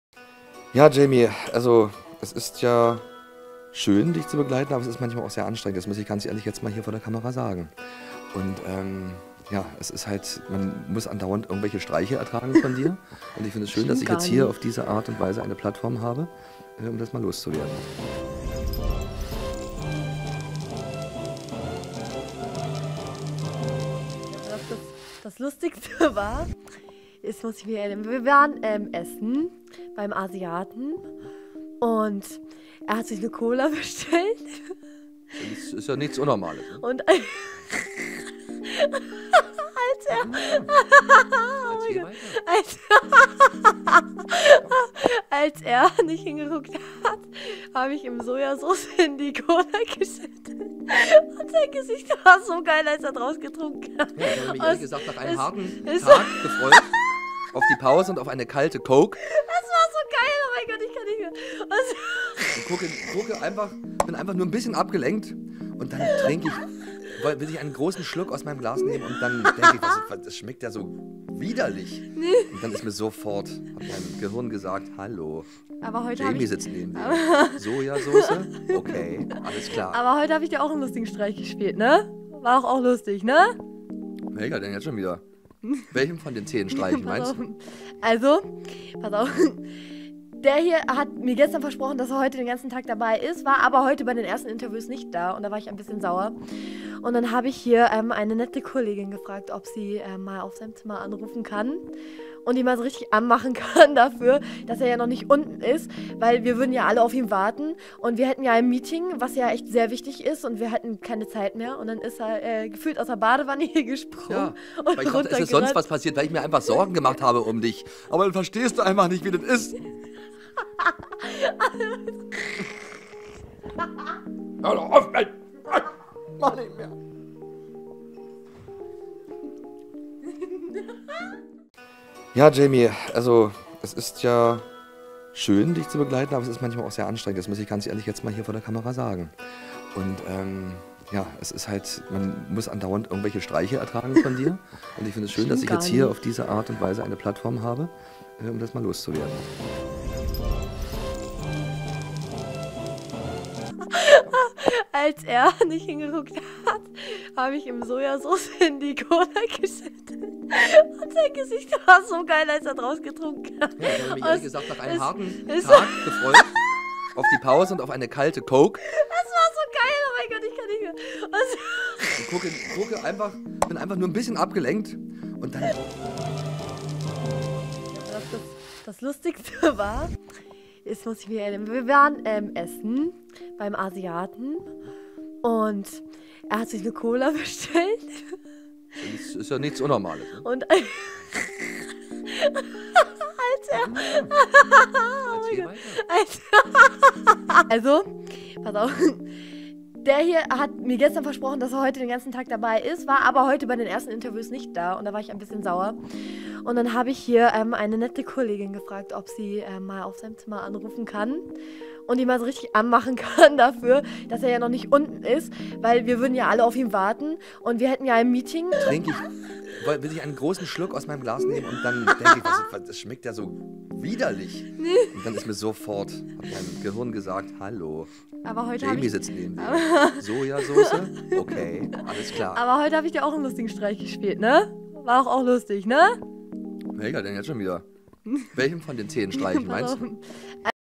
Jamie Lee und ihre Streiche | Eurovision Song Contest | NDR
In Stockholm wird Jamie-Lee permanent von Bürger Lars Dietrich begleitet. Ein entspannter Job für den Sänger, Comedian und Moderatoren? Denkste, bei .

Jamie-Lee beeindruckt nicht nur duch Stimmgewalt und fantasievolle Outfits, sondern auch durch ihre Nervenstärke. Proben, Interviews, roter Teppich - kein .

Jamie-Lee redet nicht lange um den heißen Brei: Natürlich sei sie traurig, dass sie beim Eurovision Song Contest auf dem letzten Platz gelandet ist.

Jamie-Lee Kriewitz stellt sich persönlich in ihrer Postcard vor. Die 17-Jährige erzählt von ihren verrückten Outfits, von Glücksbringern und Erwartungen an den .